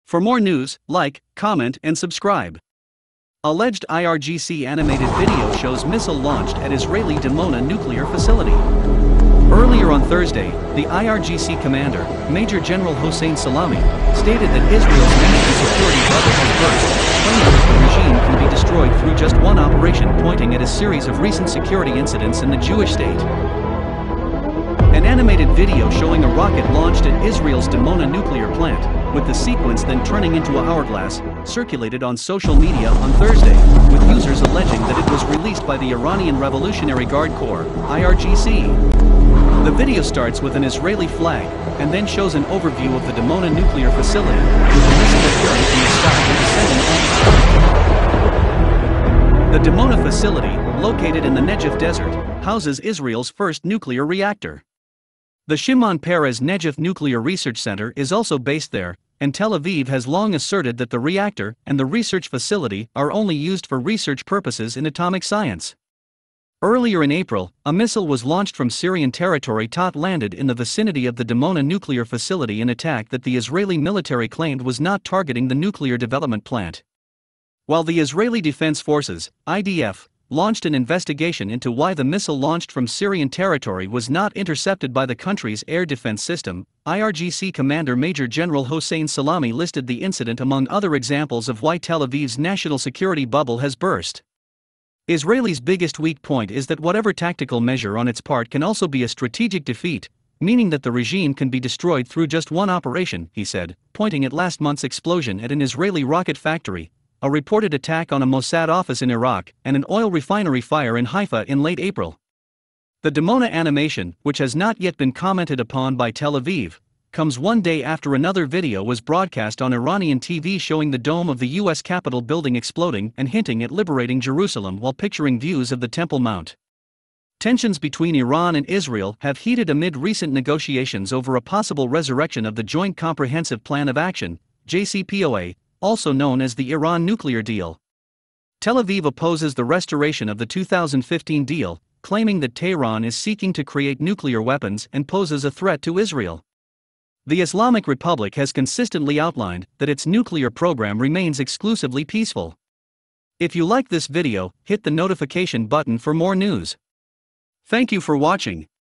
IRGC Missile strike Israeli Dimona Nuclear Facility (Animated Video)
Alleged IRGC Animated Video Shows Missile Launched at Israeli Dimona Nuclear Facility.